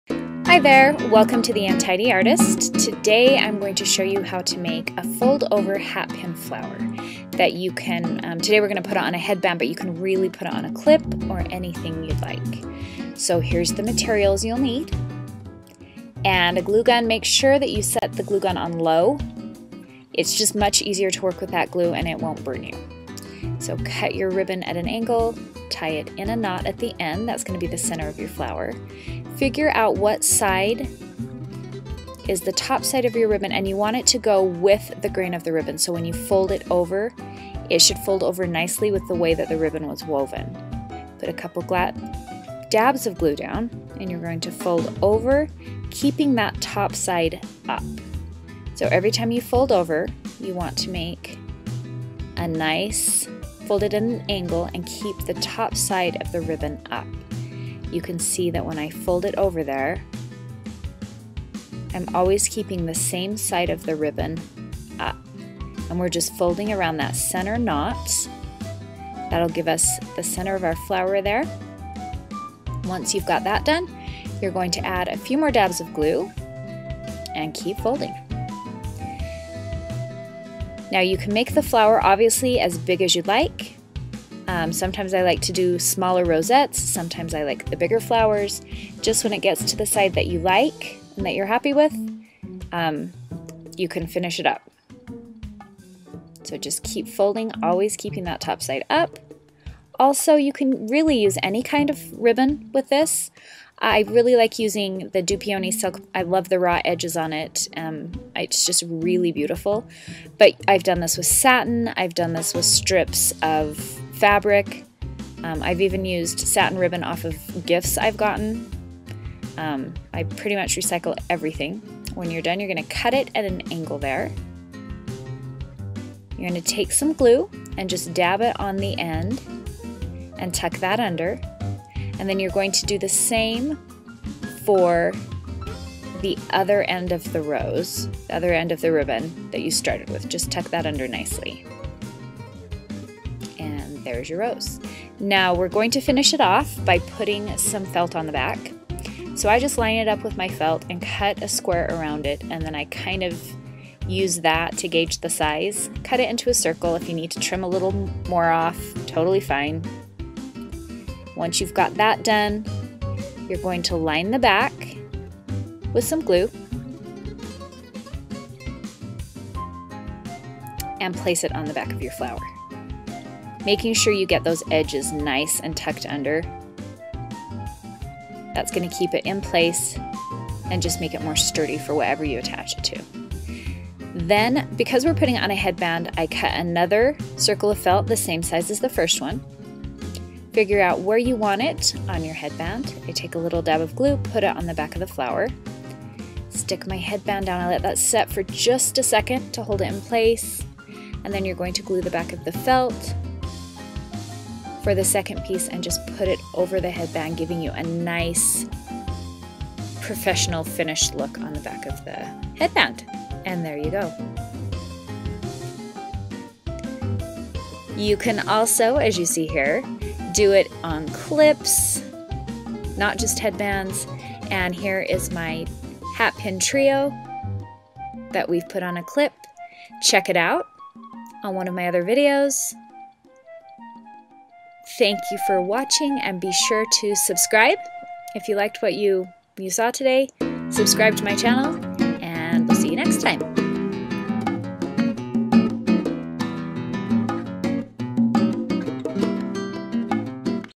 Hair Flower - Custom Silk Hat Pin Rose Headband - By Untidy Artist
Gorgeous hat pin rose hair flower on a metal band.  Takes only a few minutes to make.

Thanks Kevin!